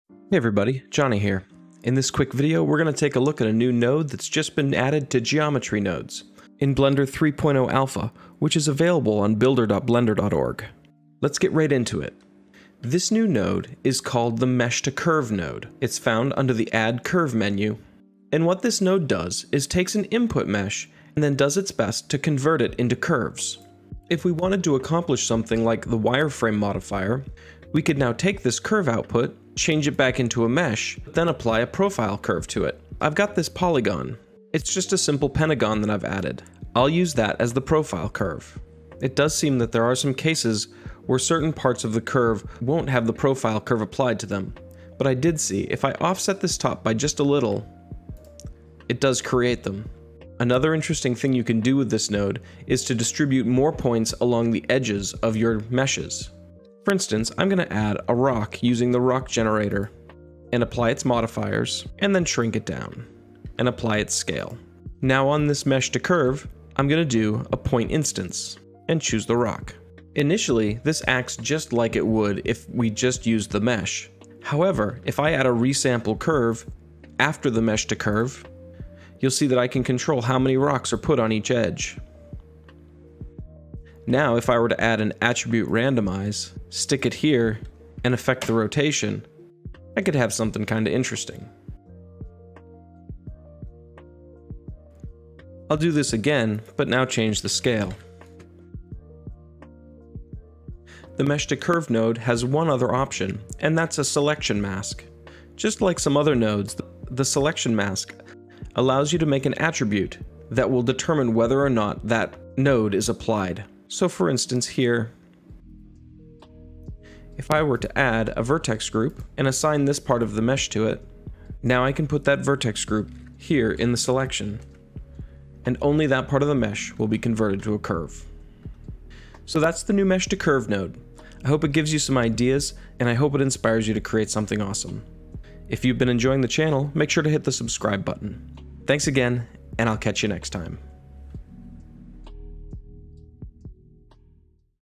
New Blender 3.0 Feature: Mesh to Curve Geometry Node
NEW!!! In this video, we will take a look at the Mesh to Curve Node which is available in the Blender 3.0 alpha build as of the May 29th, 2021 build This node lets you convert mesh objects to curves to drive parts of your geometry nodes.